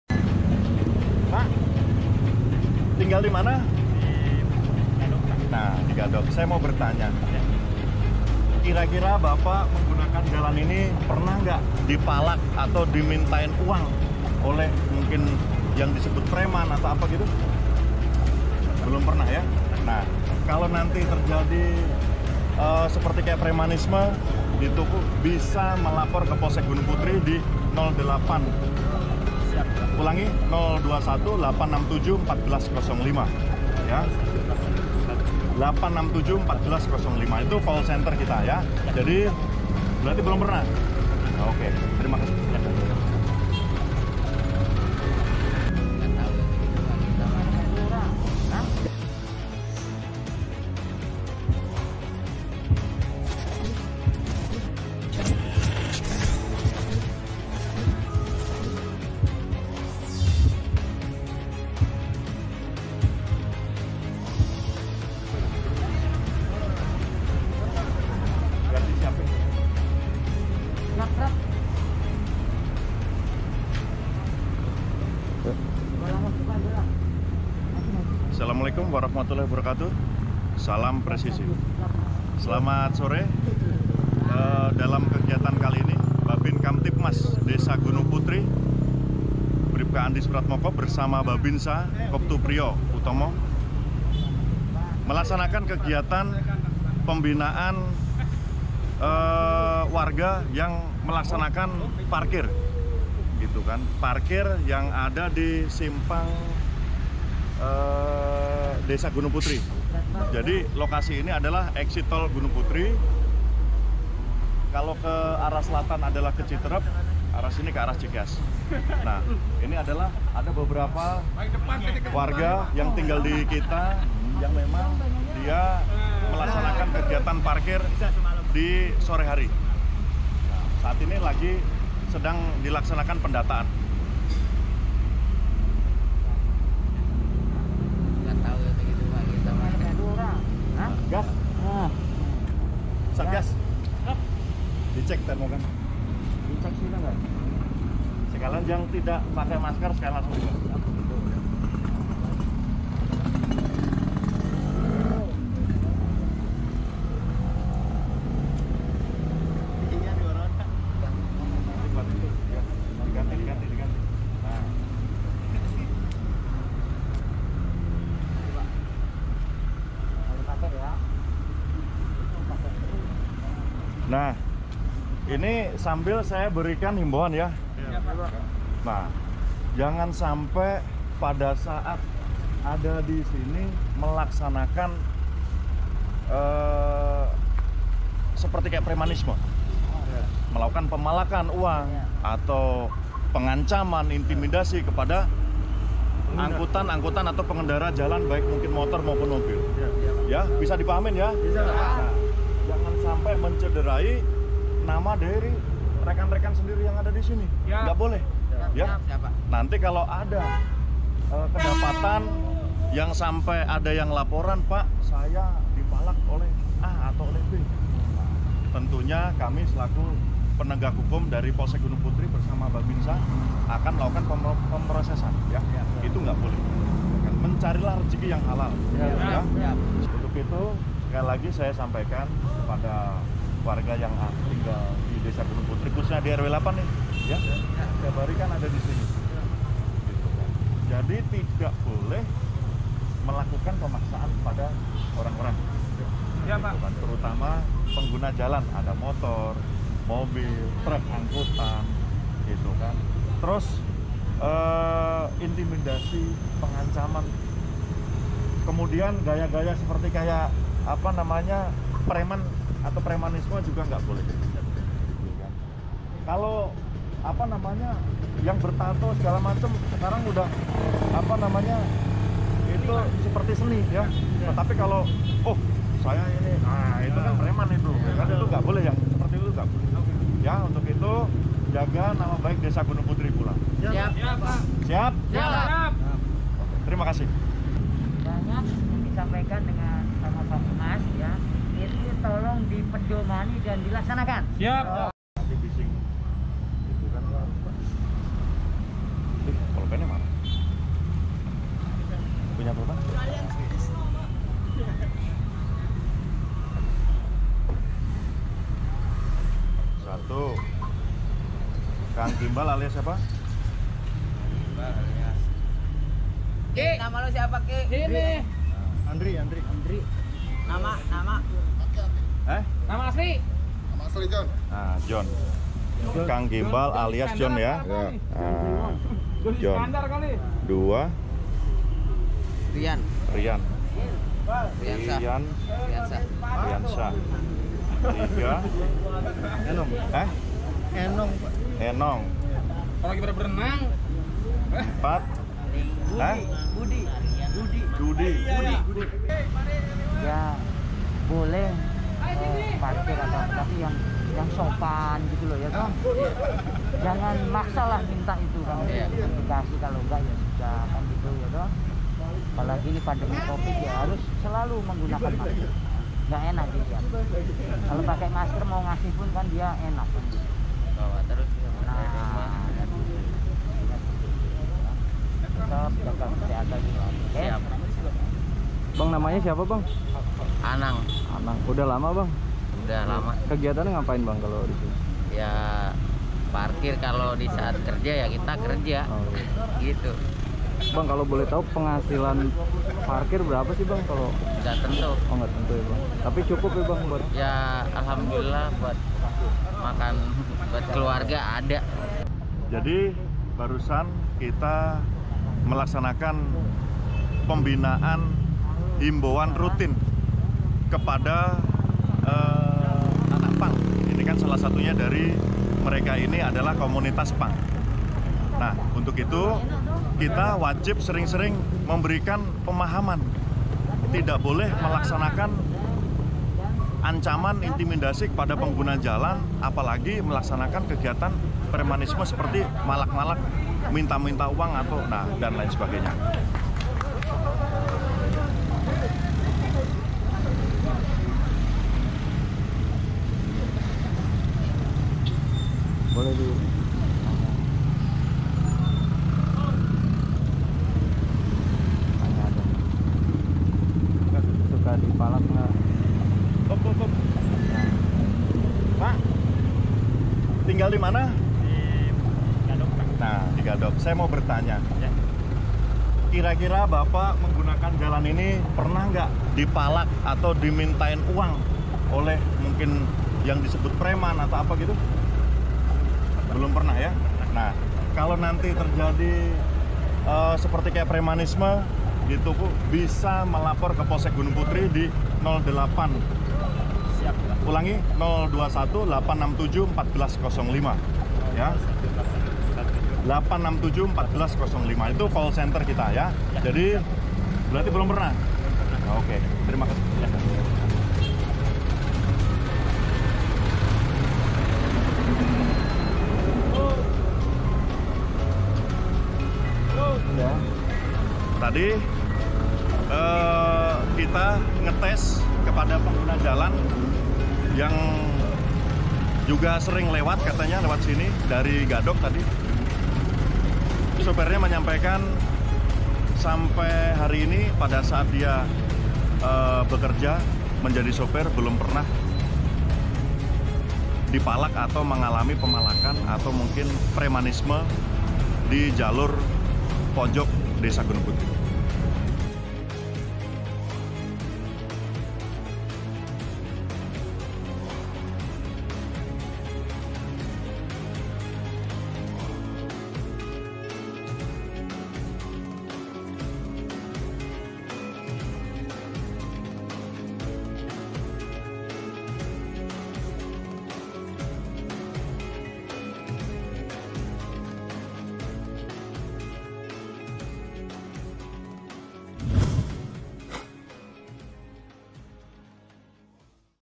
Awas Preman !
Pembinaan kepada tukang parkir oleh Bhabinkamtibmas dan Babinsa Ds Gunung putri di Simpang Exit Tol